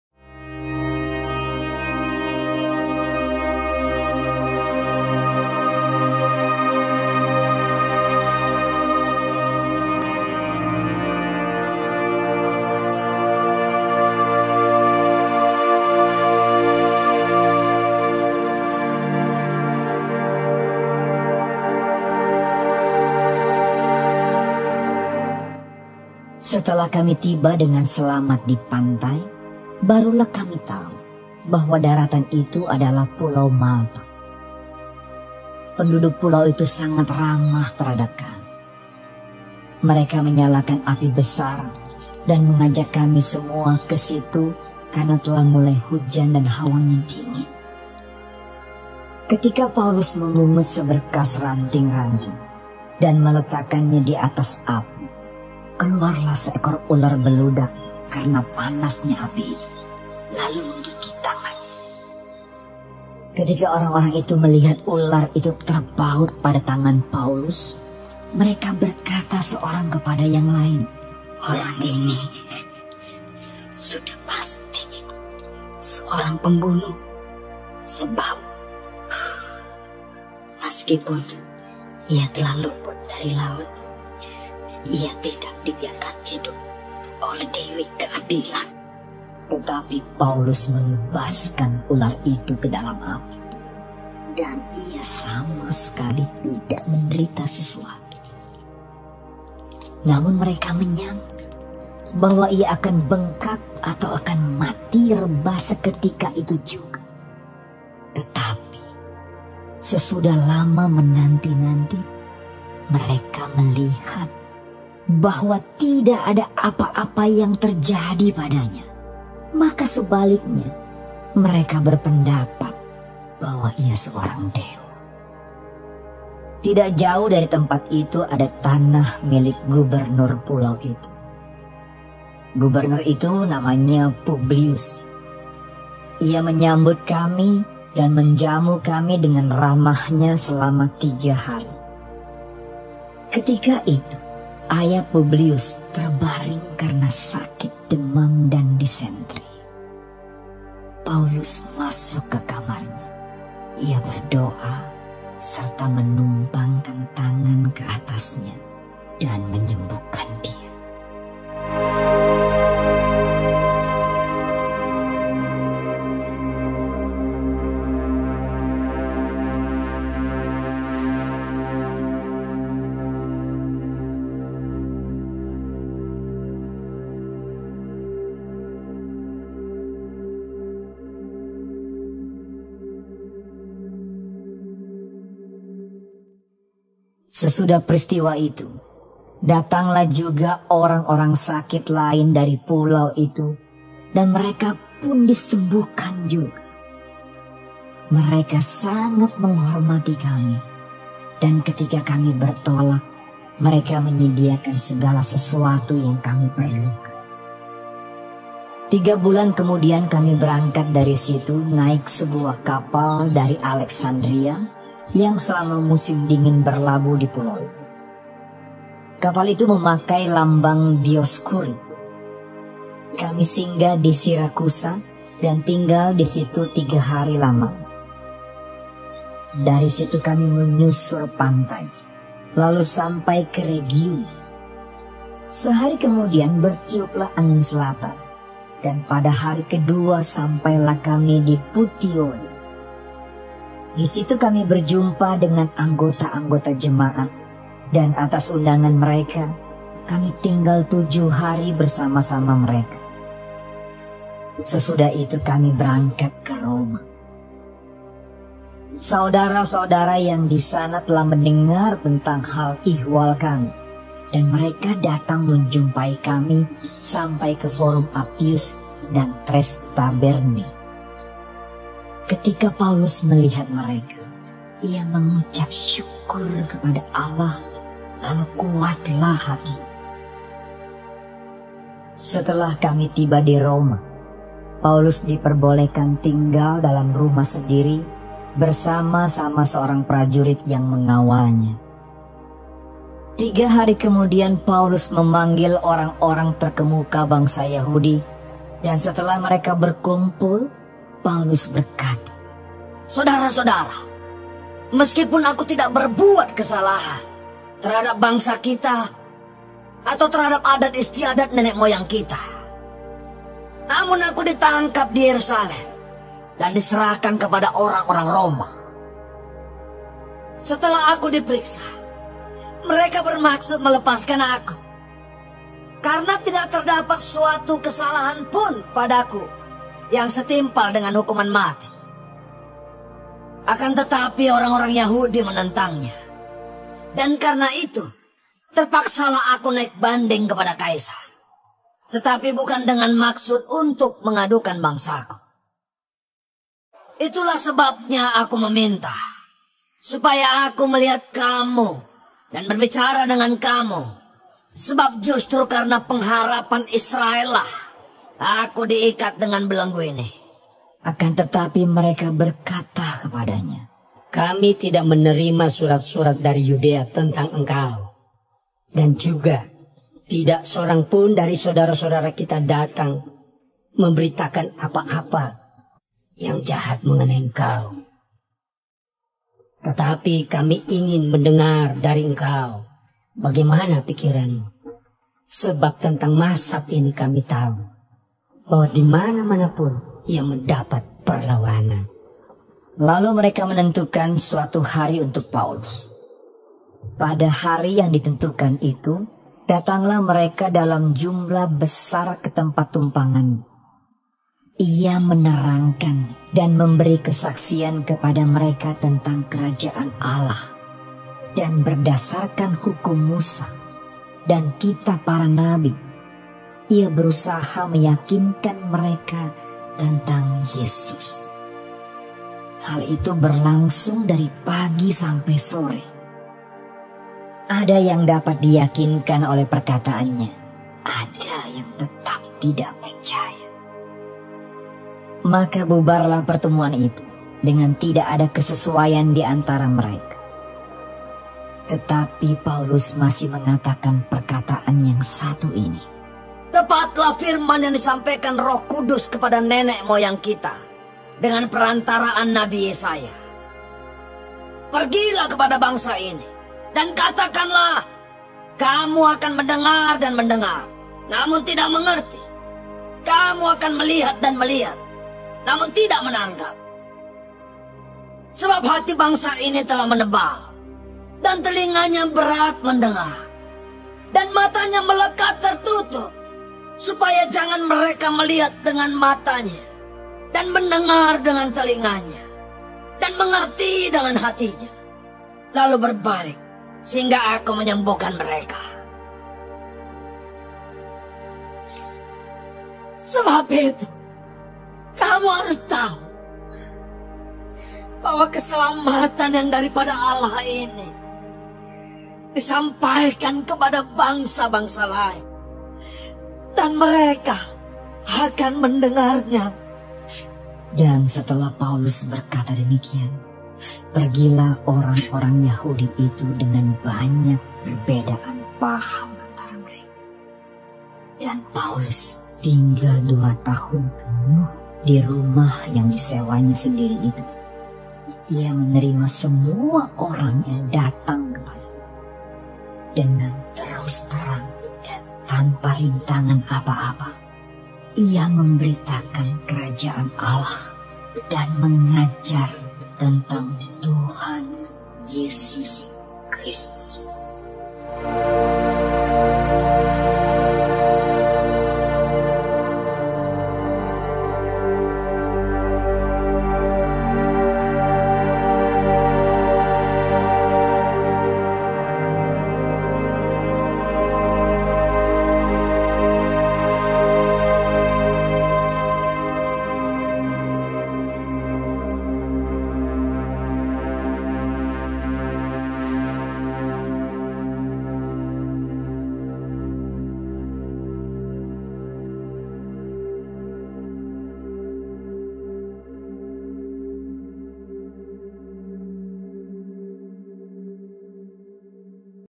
Kisah para rasul 28
Paulus memberitakan Kerajaan Allah di Roma (kisah para rasul 28)